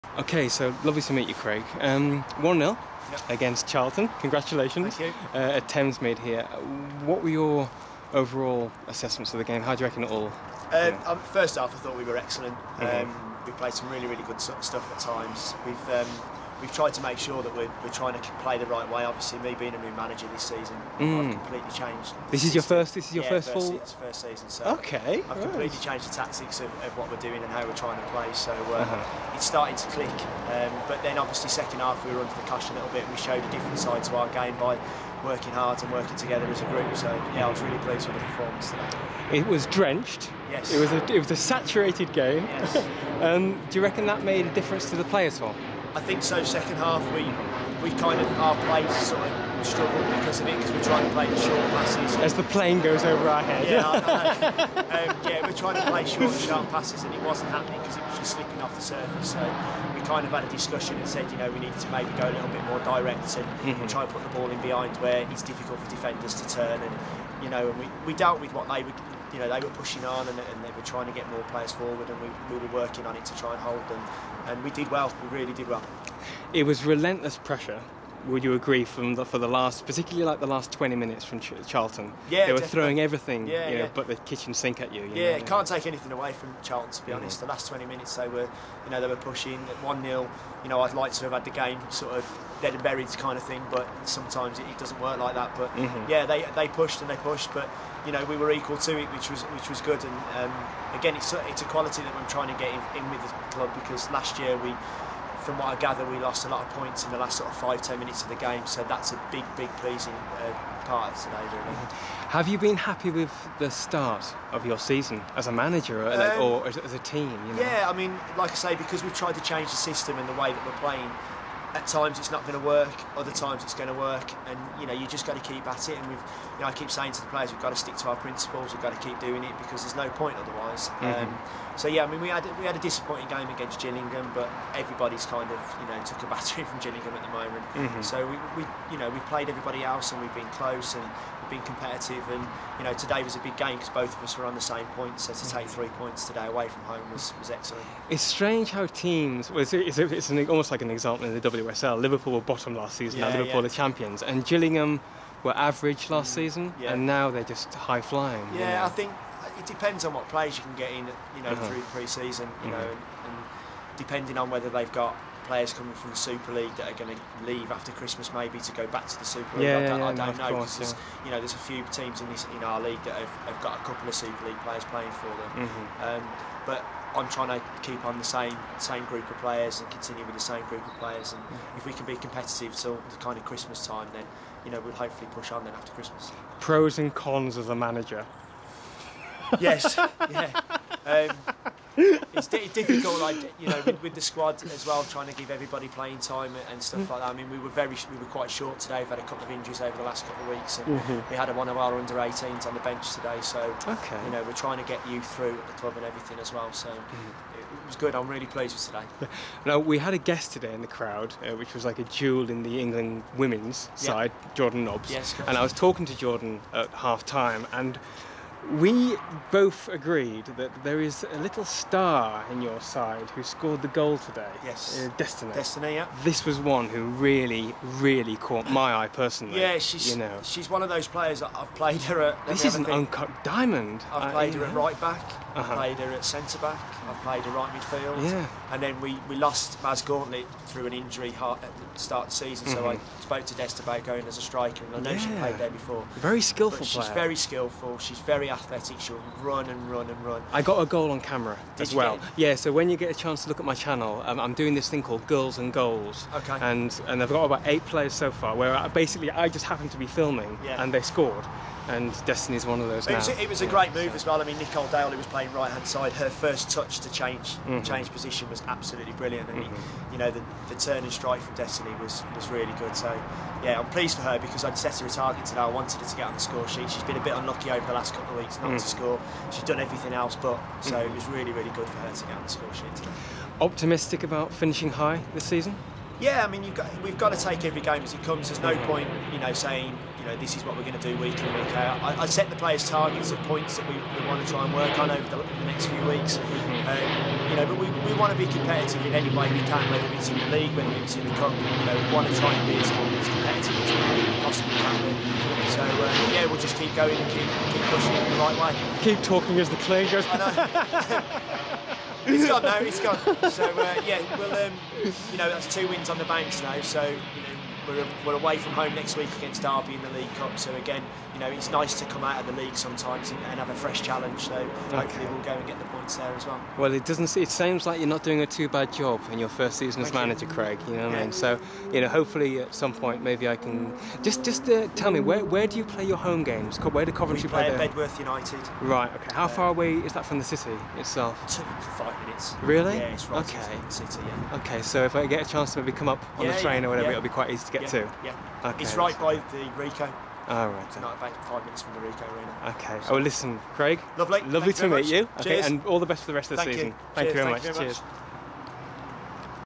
WFD - Full Time Thoughts 2013 Ft. Craig Nicholls. Coventry City Ladies Manager
Women's Football Diaries - Full Time Thoughts

An Exclusive Recorded Conversation With Coventry City Ladies Manager Craig Nicholls .We Talk About The Premier League Clash At Charlton Athletic.Recorded At Sporting Club Thamesmead London,On Sunday The 20th Of October 2013
Final Score
Charlton 0
Coventry 1